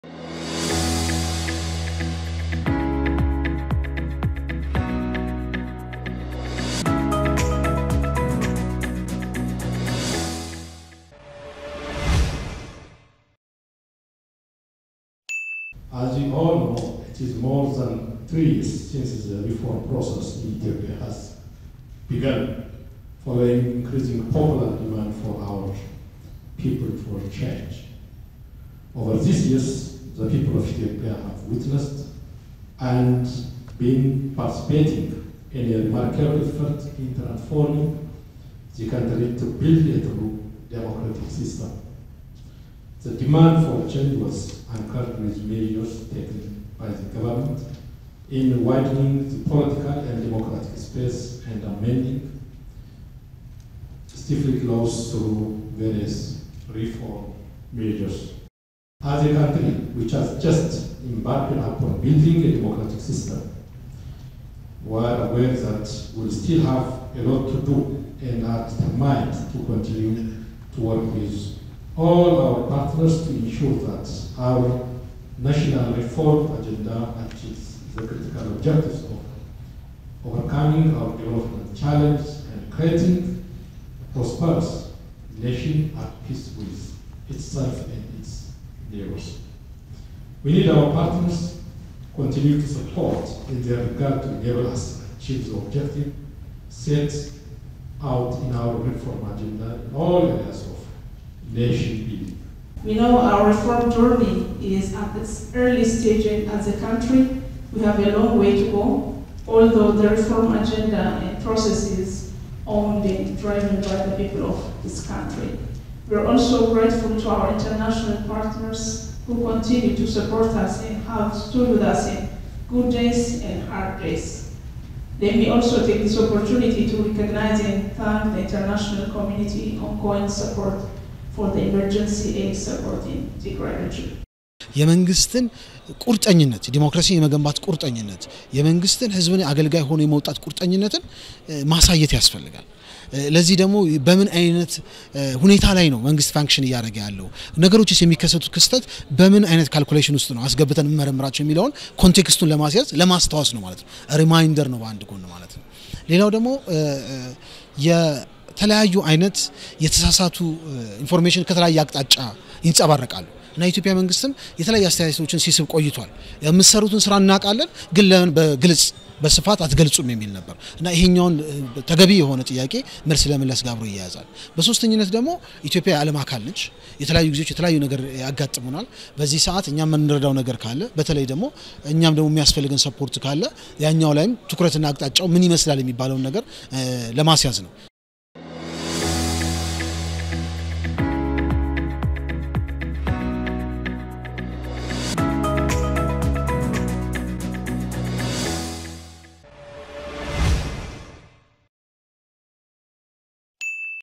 Int'l Diplomatic Community Briefed About Ethiopia's Democratic Transition
Members of the international diplomatic community based in Addis Ababa were briefed about the democratic transition process being undertaken in Ethiopia since the last three years.